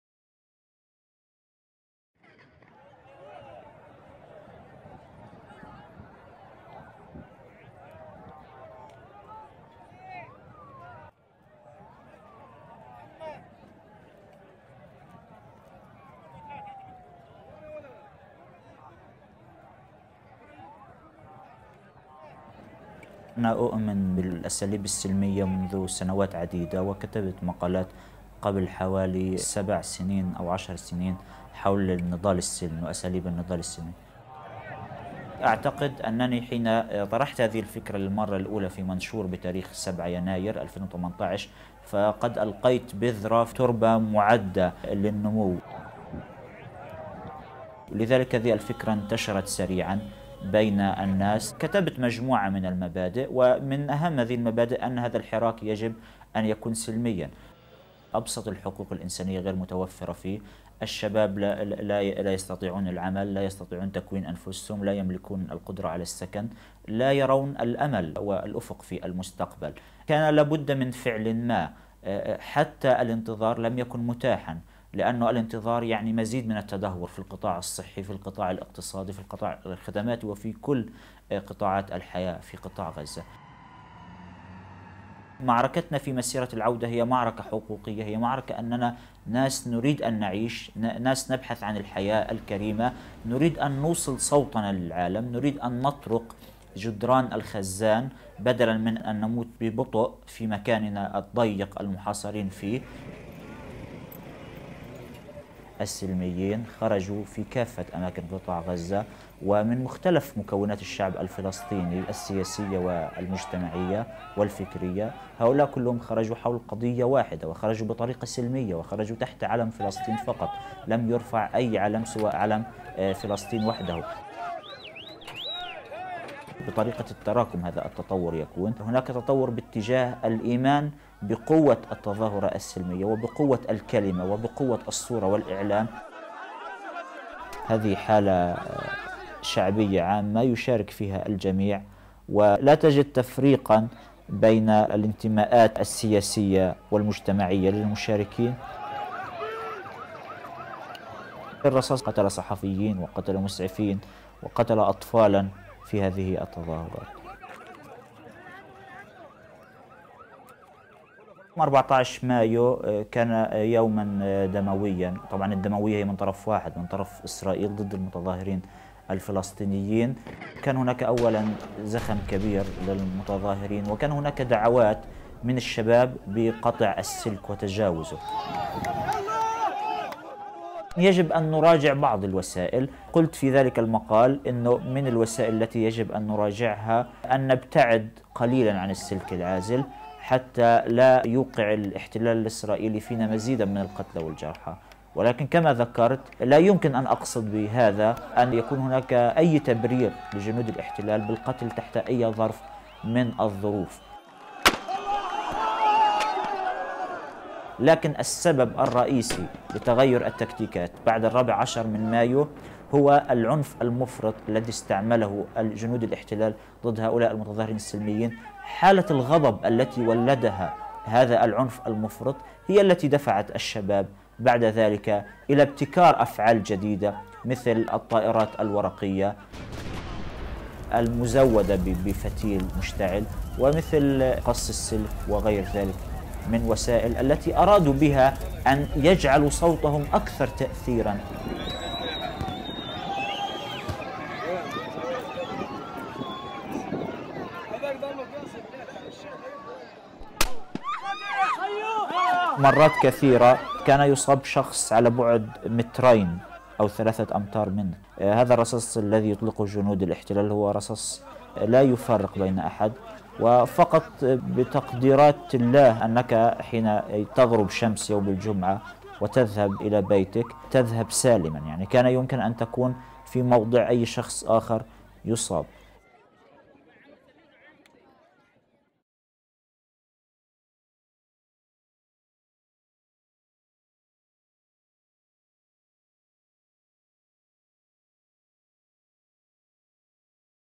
COI on Gaza Protests: Ahmad Abu Artema: the Reasons
Ahmed Abu Artema, a poet and journalist in Gaza, is one of the initiators of the protests by the separation fence to Israel, referred to as the Great March of Return. “I have believed in peaceful resistance for many years. When I proposed the idea, it was like planting a seed in fertile ground. I wrote principles, the most important that this movement must be peaceful. We don’t have the most basic human rights in Gaza. Something had to be done, we couldn’t wait any longer.”